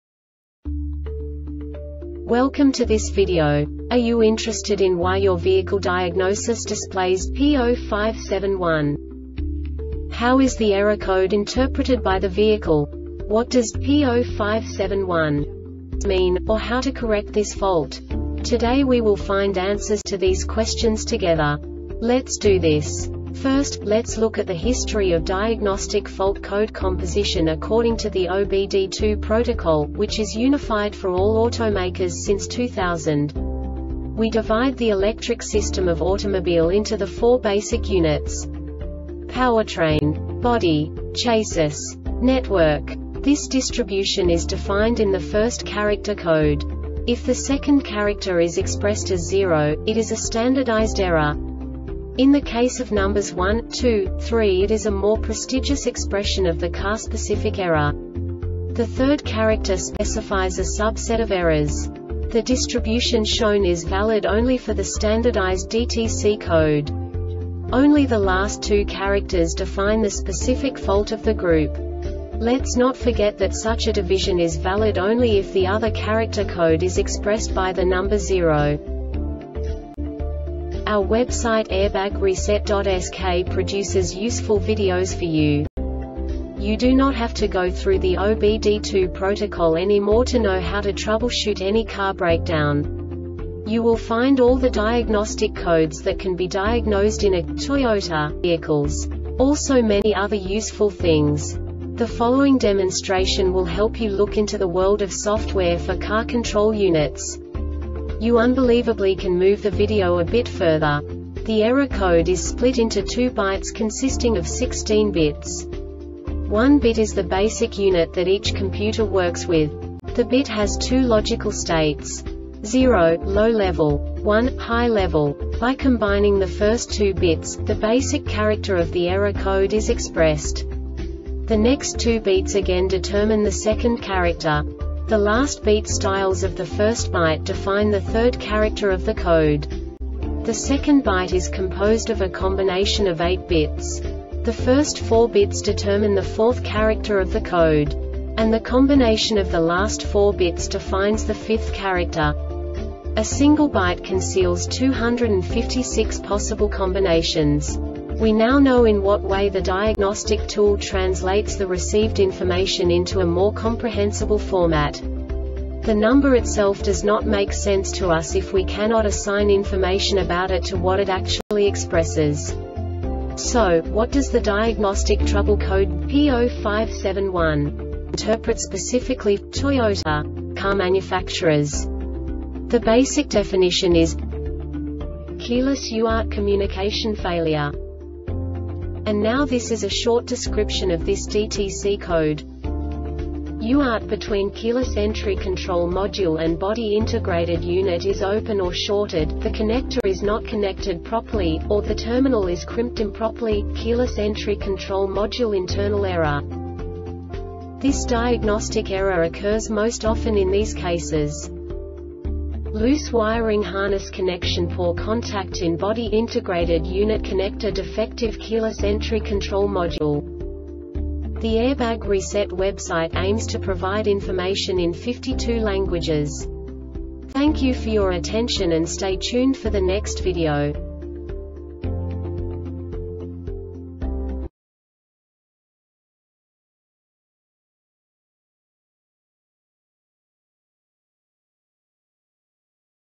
DTC Toyota P0571 Short Explanation
Contents:
0:21 Basic DTC analysis according to OBD2 protocol standard.
1:48 Insight into programming
2:58 A diagnostically understandable description of the Diagnostic Trouble Code
3:05 Basic error definition

Make:
Toyota
Code:
P0571
Definition:
Keyless UART Communication Failure
Description:
UART between keyless entry control module and body integrated unit is open or shorted,the connector is not connected properly,or the terminal is crimped improperly,keyless entry control module internal error.
Cause:
Loose wiring harness connectionPoor contact in Body Integrated Unit connectorDefective keyless entry control module